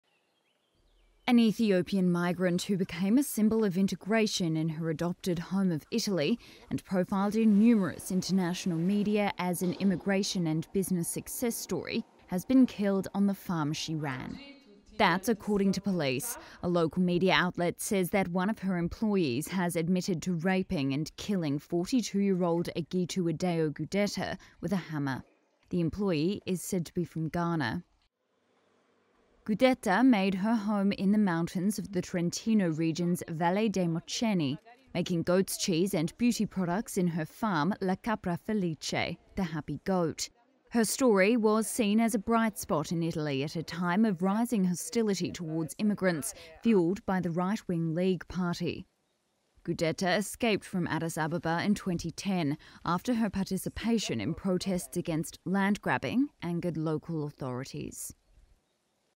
Ethiopian migrant who became symbol of integration in Italy killed on her farm
An Ethiopian migrant who became a symbol of integration in Italy, her adopted home, has been killed on her farm where she raised goats for her cheese business, police said on Wednesday.